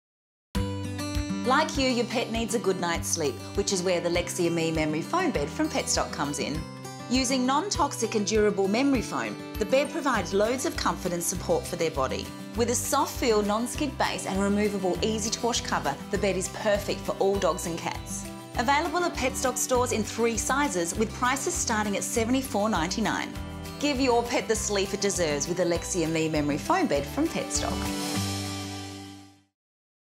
How to choose the right bed for your dog
In the new PETstock Petsmarts TV ad campaign, Pooches at Play TV Host and Dog Behaviourist, Lara Shannon, tells us why she chooses the Lexi & Me memory foam bed for her dog Darcy.